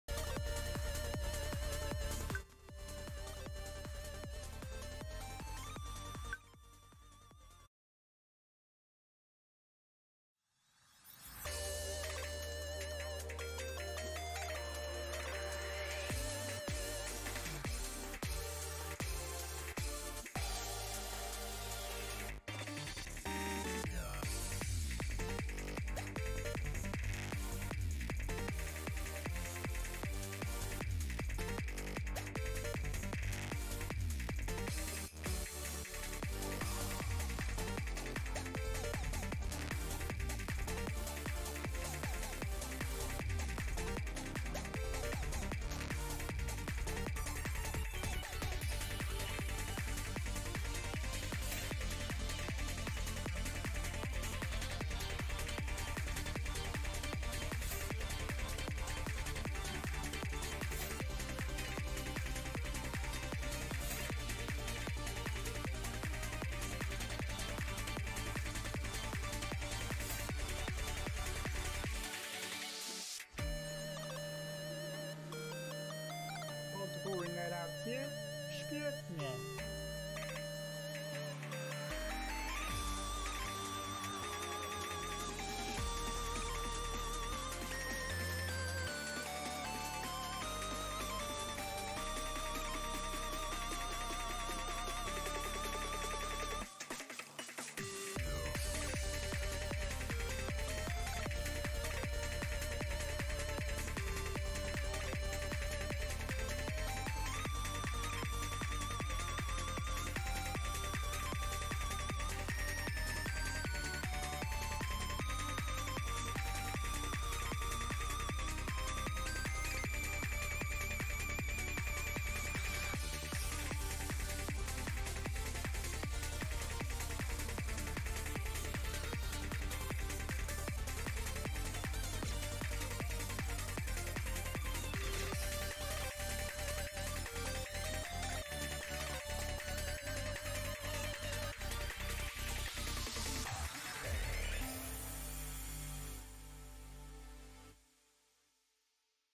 2020 HK OWC Tryouts HR Pool FC Choke
hard map

to put into perspective, top score in tryouts with v2 was 434k (~300-400x combo)